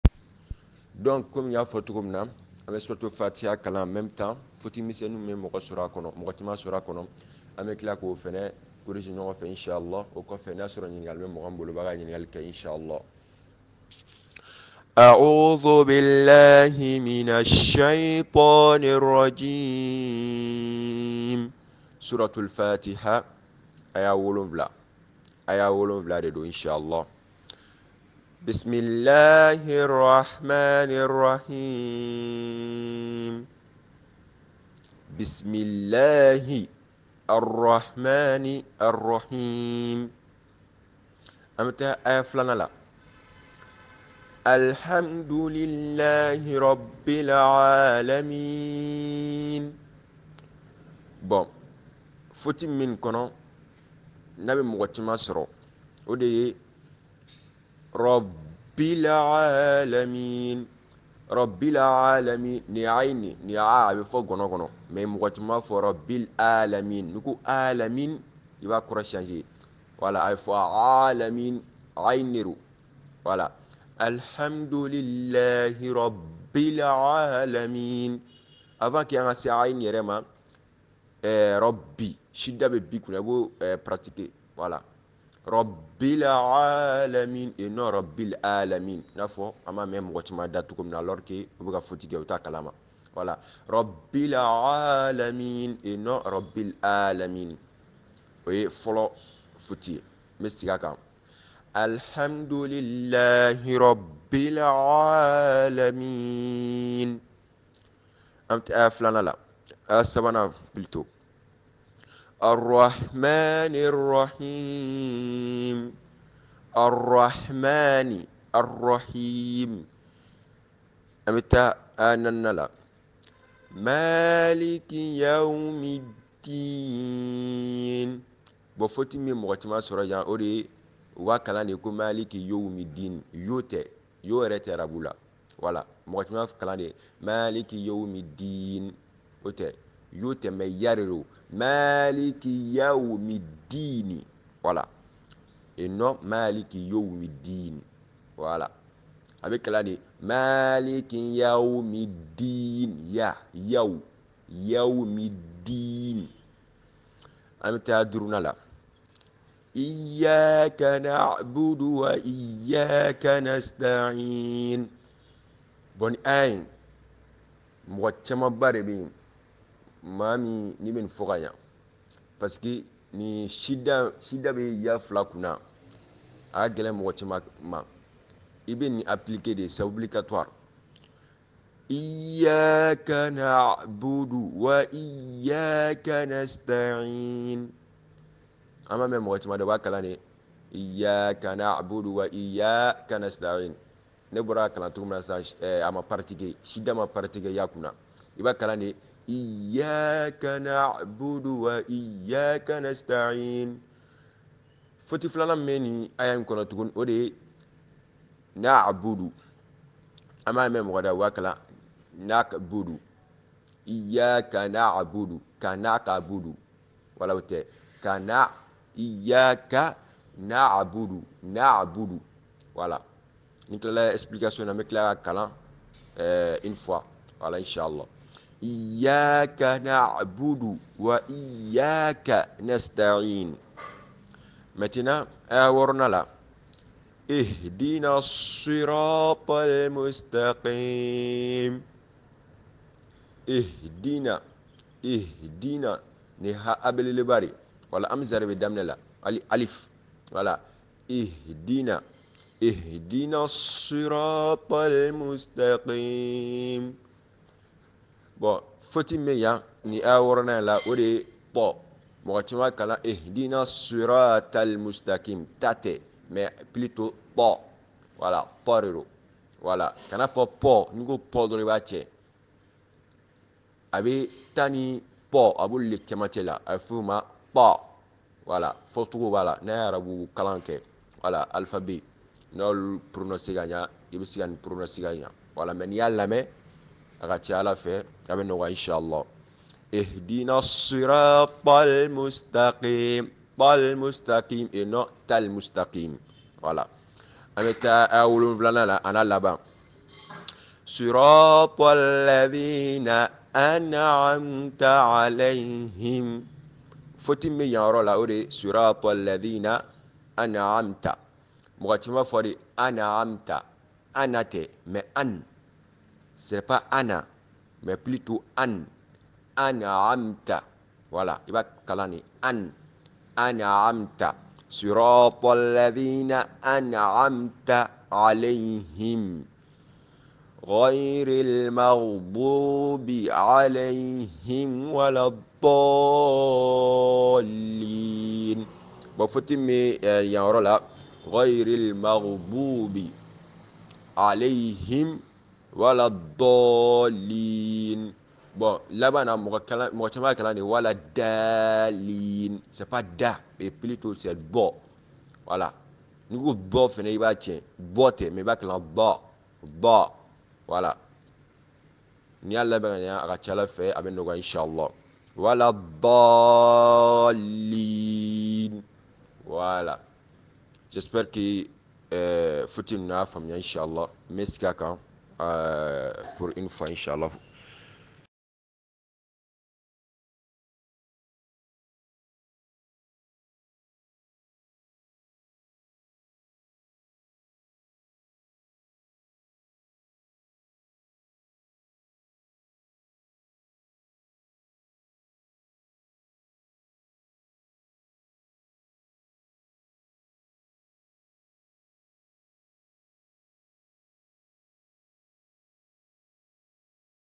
Apprendre à reciter la sourate AL-FATIHA sans ERREURS
La récitation du coran dans prière est une obligation qui incombe à chaque musulman.
Ce nous faisons quotidiennement des erreurs dans la récitation de certaine sourate quotidienne du coran.
C'est cours on pour objectifs d'aider nos frères musulmans à corriger ou à apprendre comment bien reciter c'st différents sourates du coran.